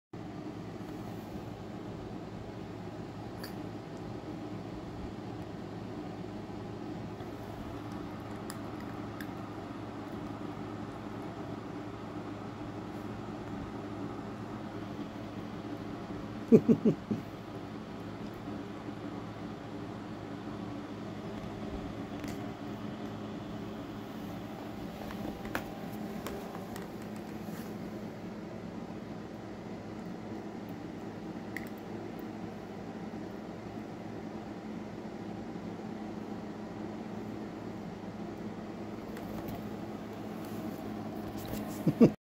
Minidog having fun eating!
our puppy eating and having fun June 30, 2022